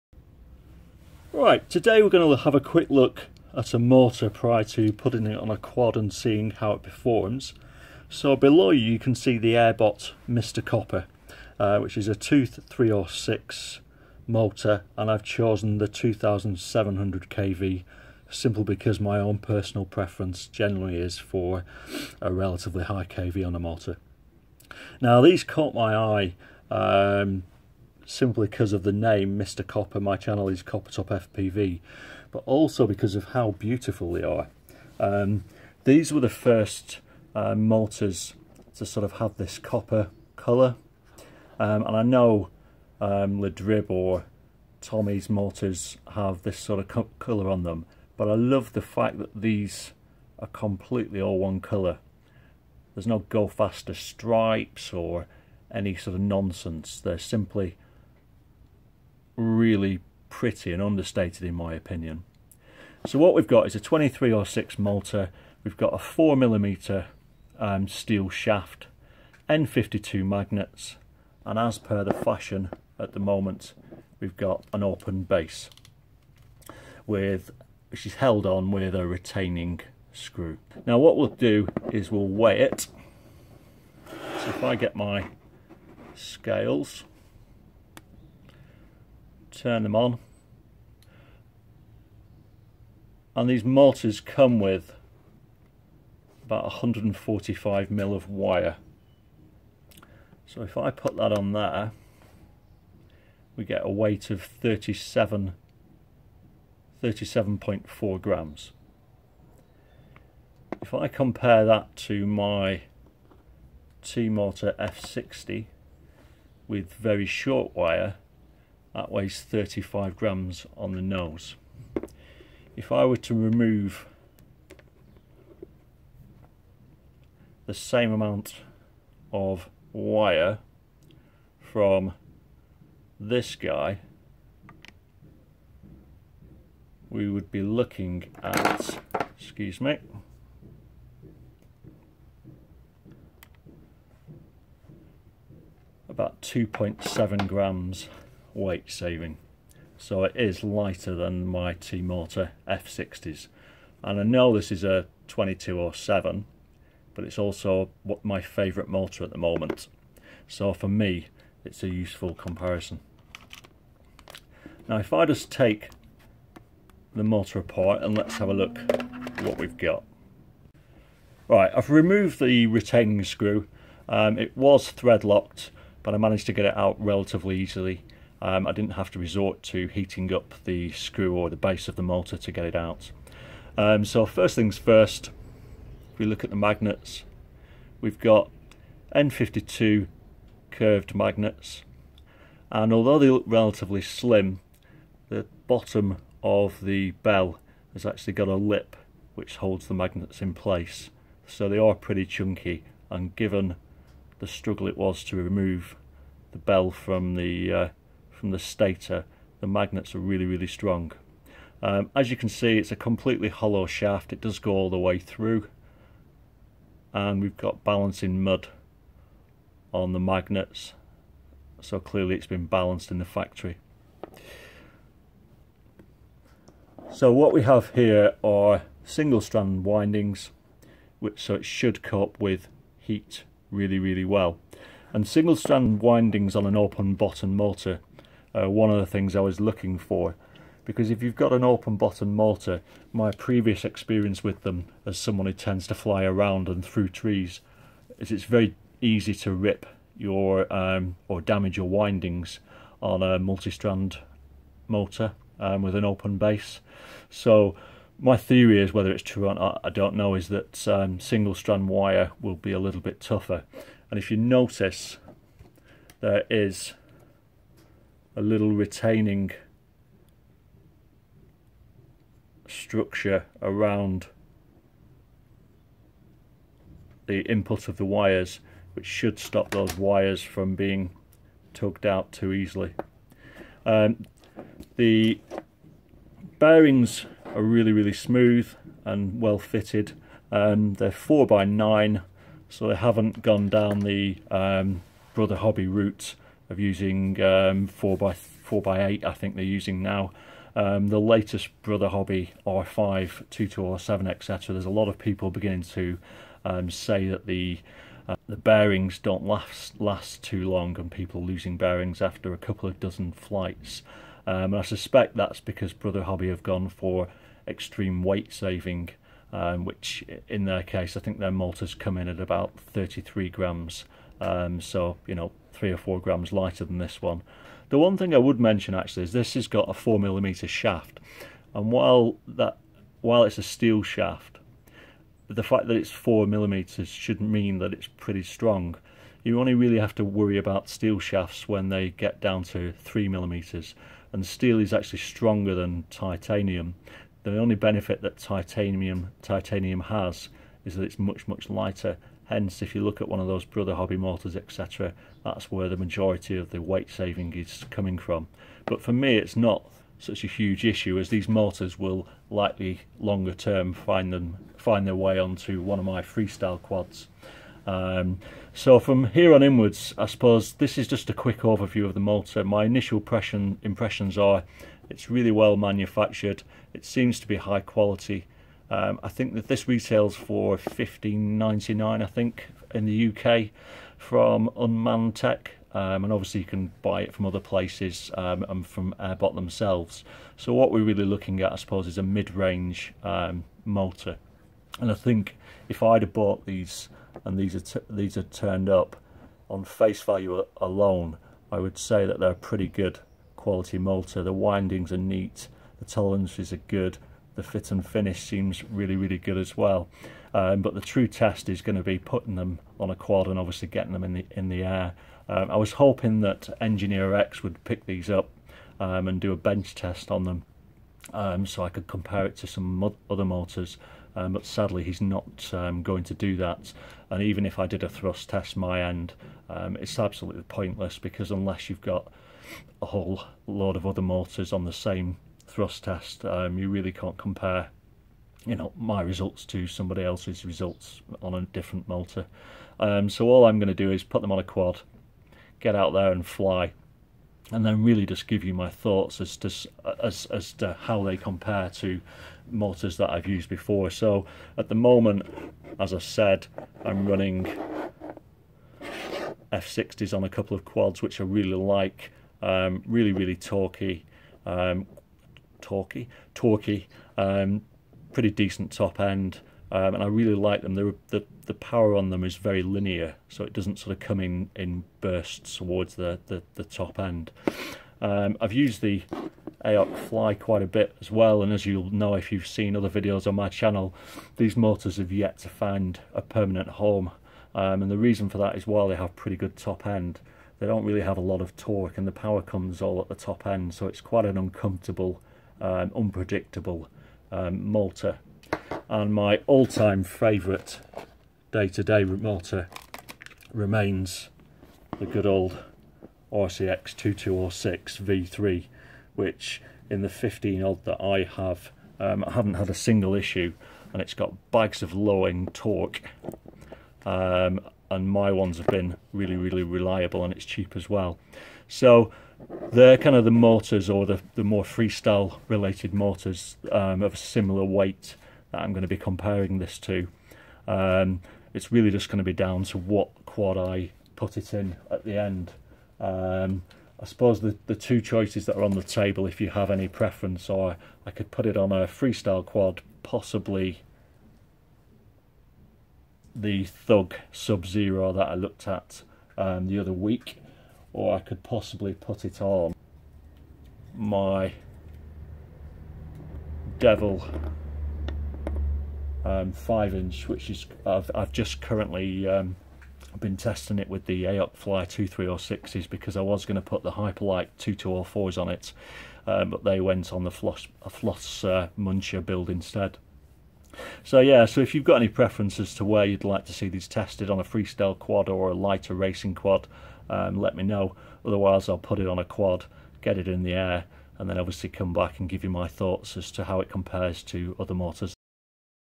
Mr Copper 2306 2700KV / Part 1 quick overview prior to flight test
The links include airbots thrust tests showing some serious numbers. Also available in 2400kv. Rated to 5s.

I since also got the 2400kv which I much prefer on a freestyle build. For these I’d recommend a really light setup. Either way I’ve found them to be sturdy, smooth,  torquey and very quick. Great motor.